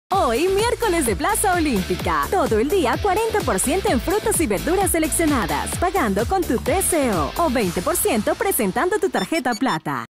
Descuentos 27 de junio
¡Hoy es Miércoles de Plaza! Frutas y verduras referencias seleccionadas con el 40%dcto pagando con TCO ó 20%dcto presentando tu Tarjeta Plata. Válido 27 de junio *AC